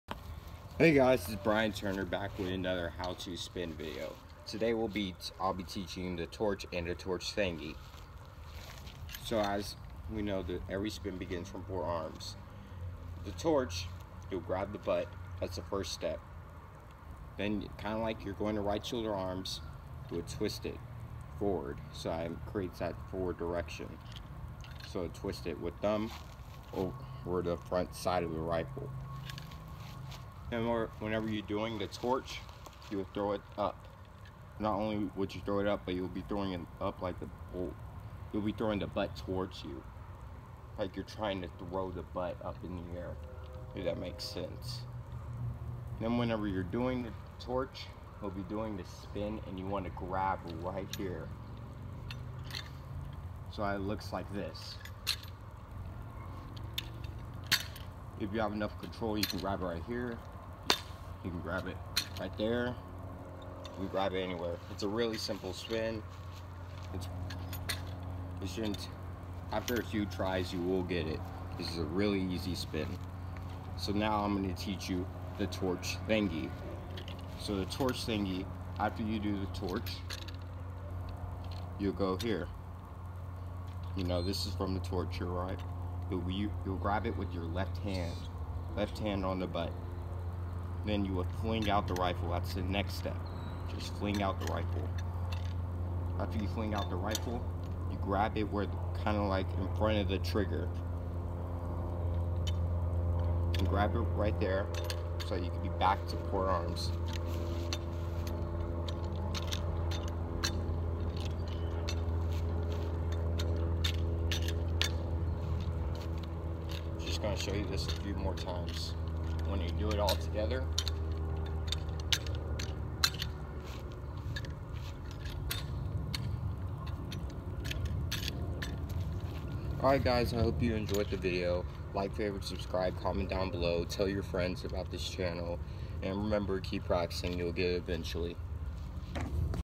Episode 3 Arm drill exhibition for dummies (Torch and torch thingy)
Hey guys this a quick video on how to do the torch and the torch thingy